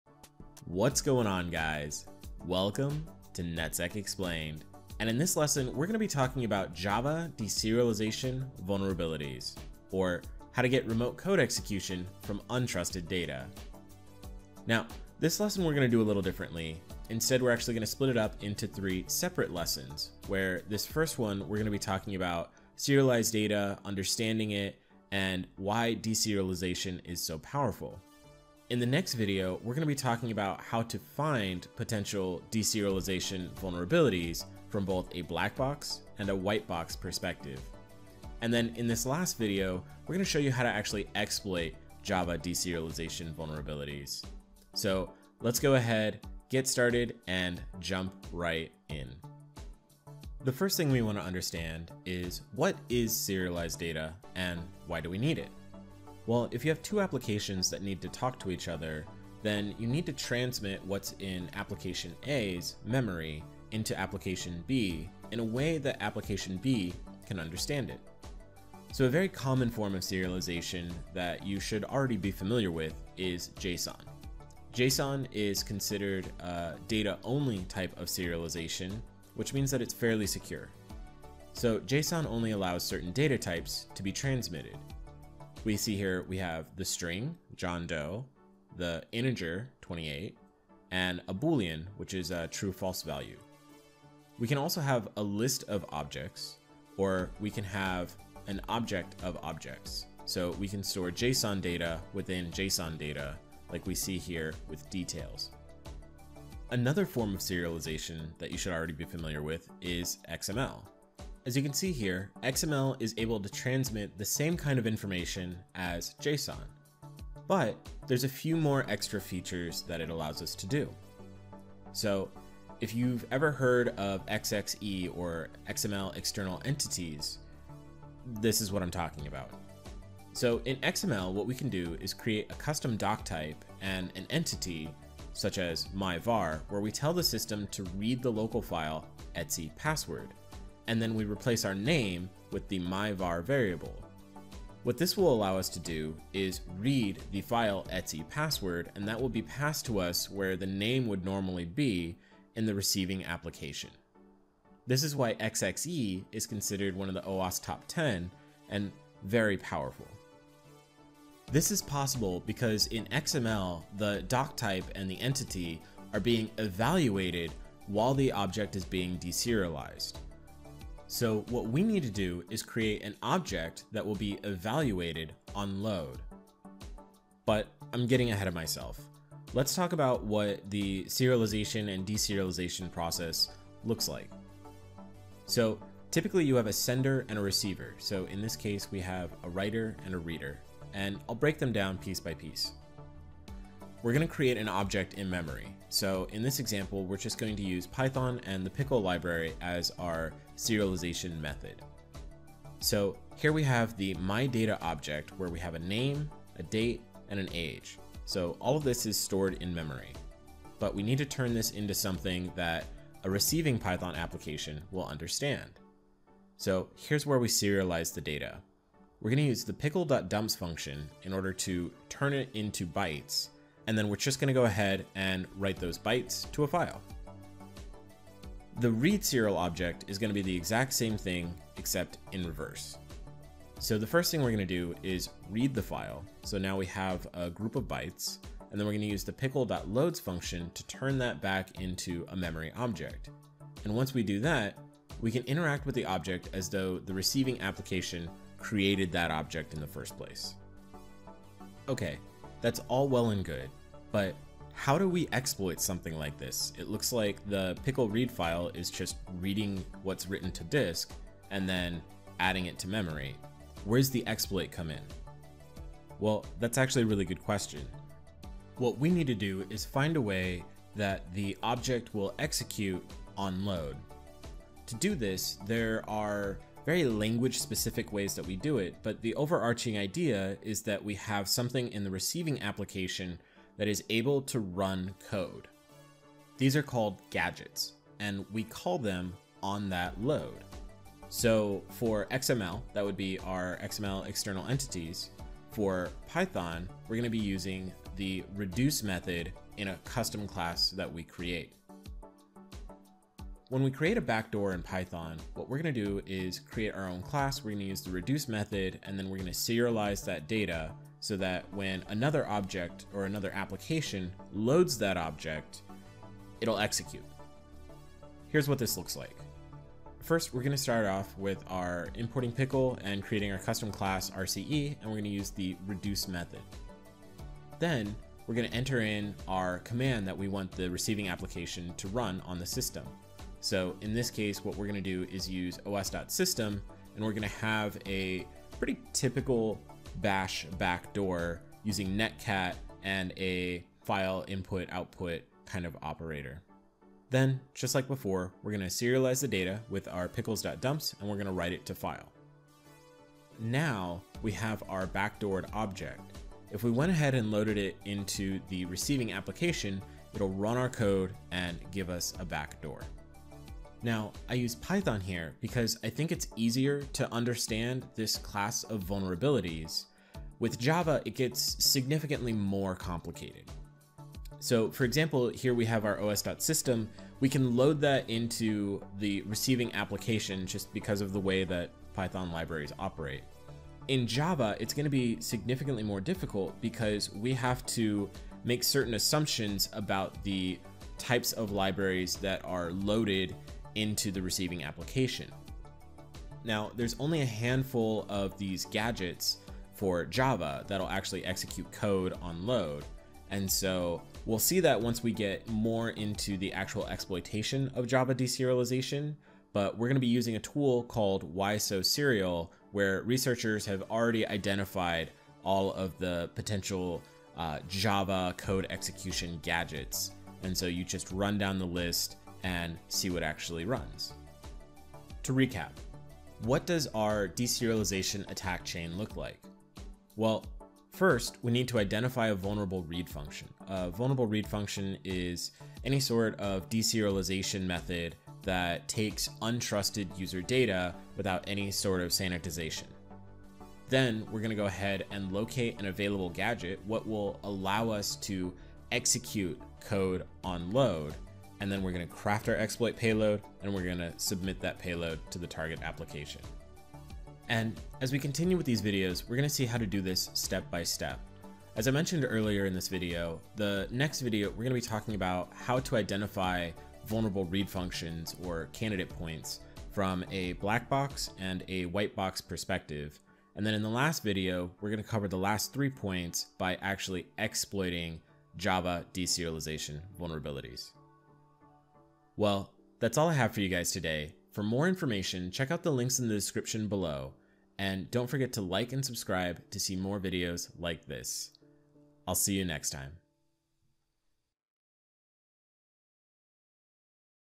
Exploit Java Deserialization | Understanding Serialized Data
This is the first video in a three part series where we dissect how to exploit Java deserialization vulnerabilities to achieve remote code execution (RCE). In this video we talk about understanding serialized data and why deserialization vulnerabilities are so powerful.